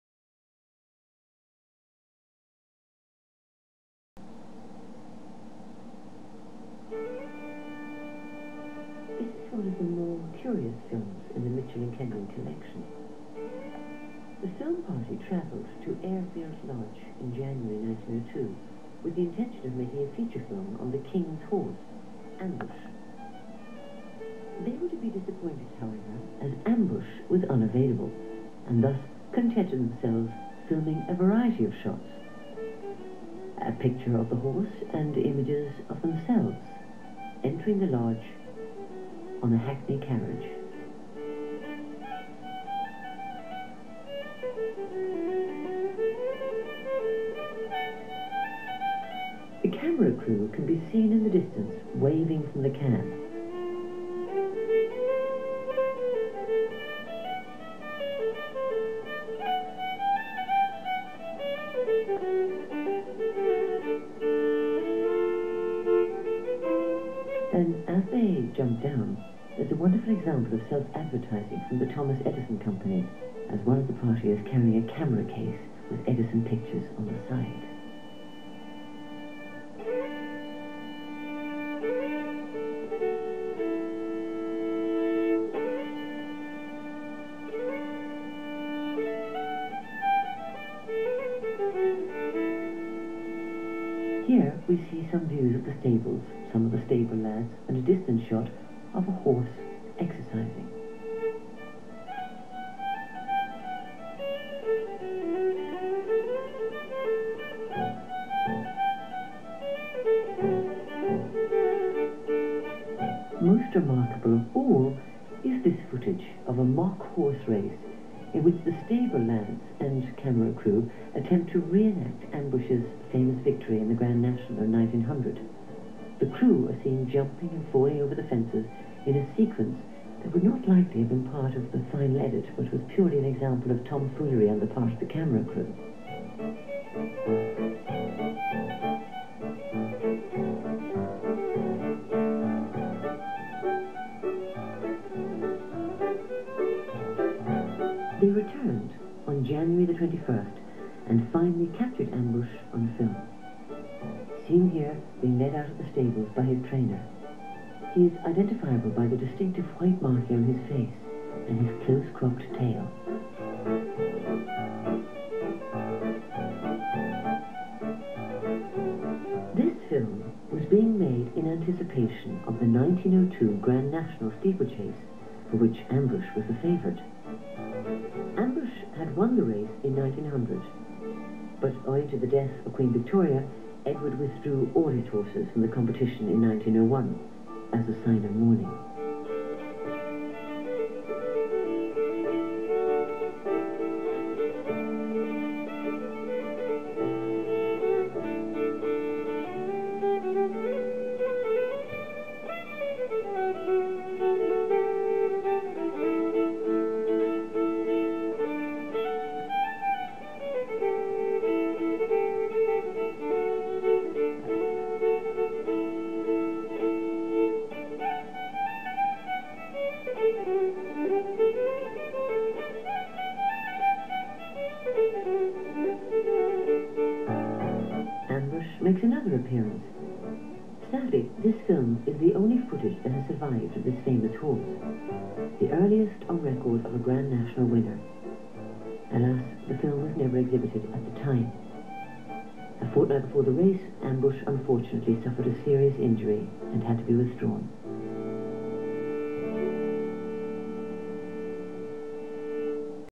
Ambush II in 1902 at Eyrefield lodge
Early film footage by Michell & Kenyon of the 1900 Grand National winner, Ambush II at the Eyrefield lodge stables.

Trained and ridden by my gt grandads ( Frederick Anthony)  cousin Algy Anthony, the Prince of Wales jockey.

Algy was born in Oxenton near Tewkesbury but grew up in Farrier st Worcester living with his grandad  a horse  breaker  - as was Algy's dad & my gt gt grandfather